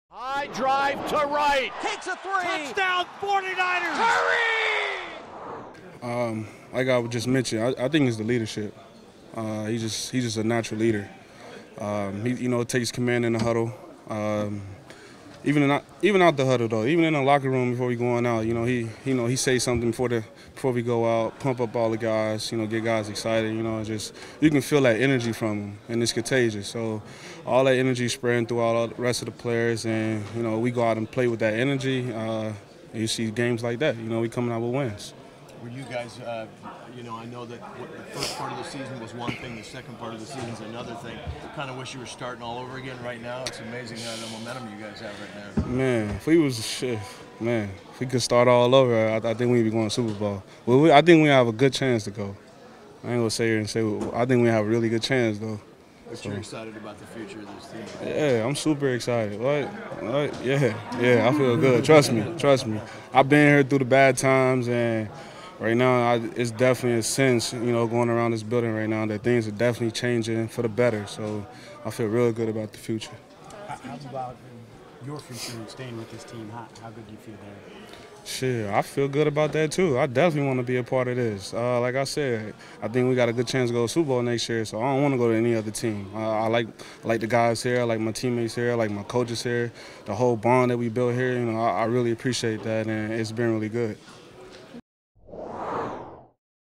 Carlos Hyde believes 49ers are a Super Bowl-caliber team
SANTA CLARA - Ask running back Carlos Hyde how far his team could go if they were start the season and he has no qualms.

“I think if we start all over we could go to the Super Bowl,” Hyde said. “I think we have a really good chance. I’ve been through the bad times, and I sense things are really changing for the better.”

That’s why Hyde, who will be a free agent at the end of the season, wants to be re-signed. Hyde has expressed that he wants to return to the 49ers, and that hasn’t changed after the 49ers’ big win over the playoff-bound Jaguars.

“I don’t want to go anywhere else,” Hyde said. “I like the guys here, I like the coaches here, I like my teammates here. We’ve got a really good bound.”

Hyde gained only 54 yards on 21 carries against a dominating defense, but he’s runs kept the play-action fake alive for quarterback Jimmy Garoppolo. The 49ers’ quarterback did the rest, completing 21 of 30 passes for 242 yards, a touchdown and an interception.